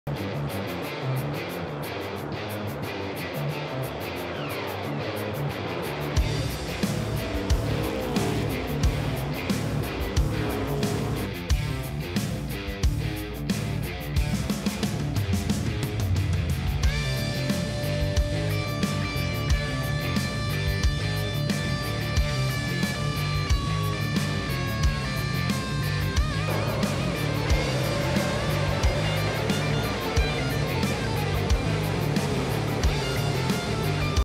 No. 19 team penalized after Bristol
NASCAR.com brings you up to speed on post-Bristol penalties.